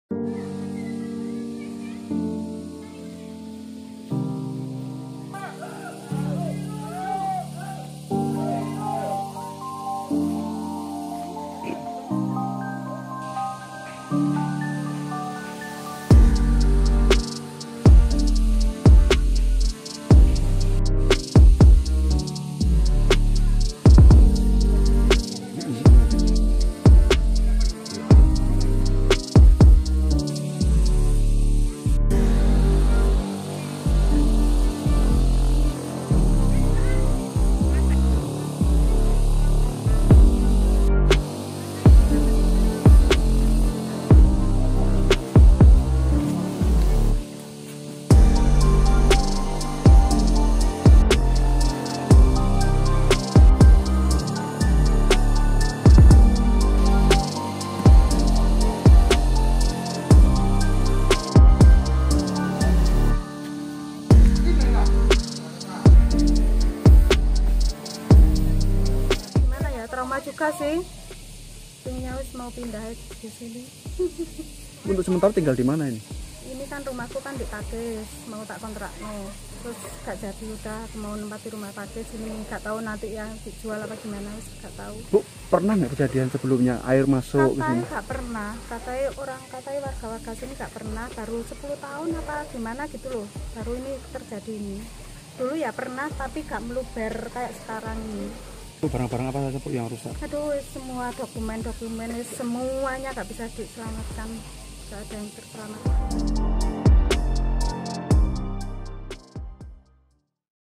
Korban Banjir Malang Trauma Untuk Kembali ke Rumah
Malang, KompasTV Jawa Timur - Hujan deras yang terjadi pada senin siang hinggga malam mengakibatkan banjir di sejumlah titik kawasan di Kota Malang. Salah satu titik yang cukup parah terdampak banjir adalah jalan Simpang Teluk bayur Kota Malang. Korban banjir mengaku trauma untuk kembali menempati rumah mereka.

Inilah kondisi rumah Warga di jalan Simpang Teluk bayur Kelurahan Pandanwangi Kecamatan Blimbing Kota Malang. Lumpur dan sampah material banjir pada senin malam masih berada di halaman rumah Warga Korban banjir. Dua rumah Warga terdampak cukup parah setelah air dari anak Sungai Bango merendam rumah mereka.

Menurut Warga Korban banjir  selain perabotan rumah yang terendam banjir, sepeda motor Warga juga tidak sempat terselamatkan karena air yang datang sangat cepat. Bahkan derasnya air yang datang juga menyeret mobil milik Anisa.

Selain kerugian material, banjir ini juga menyebabkan trauma pada Warga. Anisa bersama keluarganya mengaku tidak mau lagi menempati rumah mereka. Untuk sementara dua keluarga Korban banjir mengungsi ke tempat yang lebih aman.

Sebelumnya pada senin malam, banjir merendam kawasan permukiman. 7 jiwa terjebak di dalam rumah saat banjir menerjang satu diantaranya adalah seorang Ibu hamil. Setelah kurang lebih tiga jam, 7 Warga berhasil dievakuasi dengan menggunakan perahu karet.